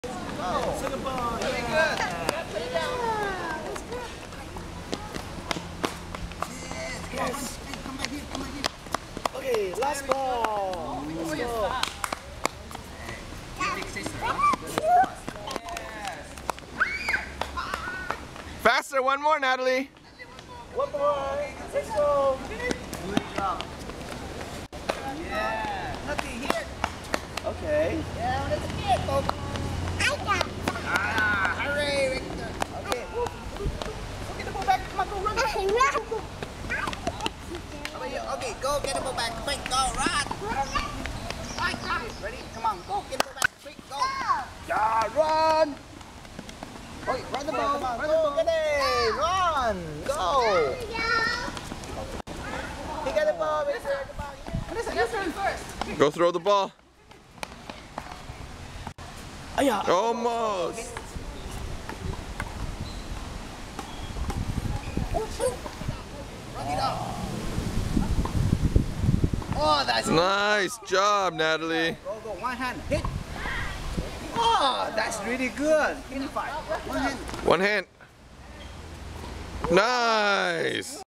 Toddler Natalie tennis lessons
Watch all the fun activities Natalie enjoyed during her very first tennis lesson with her cousins Vanessa and Victoria.   Thanks to Ba-ba Clark for taking all the videos and creating this great collage!